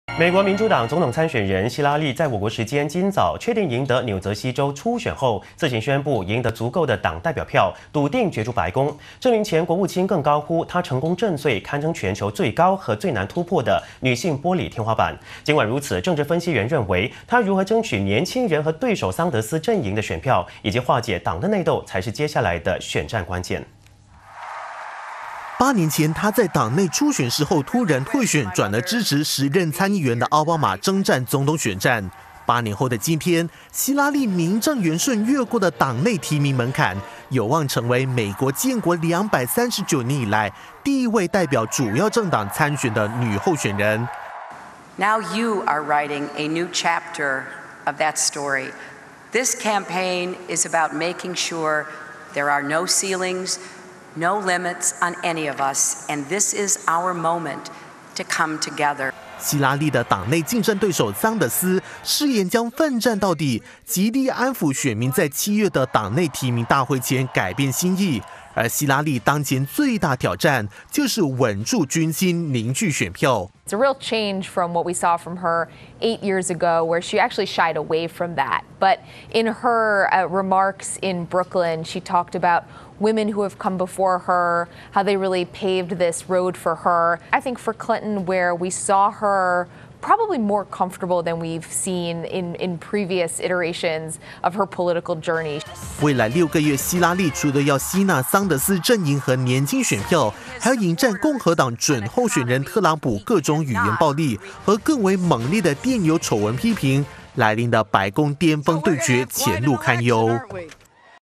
希拉丽稳拿民主党提名 震碎美国"玻璃天花板" [20160608]
美国民主党总统参选人希拉丽在我国时间今早确定赢得纽泽西州初选后，自行宣布赢得足够的党代表票，笃定角逐白宫。这名前国务卿更高呼她成功震碎堪称全球最高和最难突破的女性玻璃天花板。尽管如此，政治分析员认为她如何争取年轻人和对手桑德斯阵营的选票，以及化解党的内斗才是接下来的选战关键。